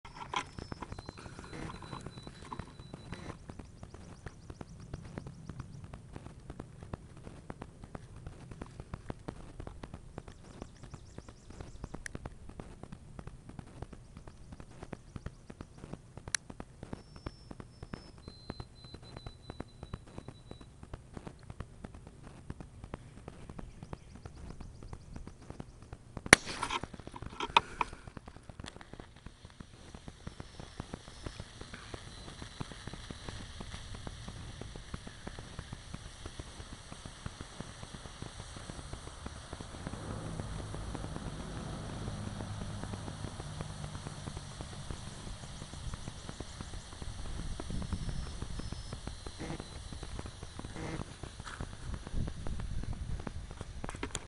AA Battery Pops
AA Batteries and car battery chargers don't mix.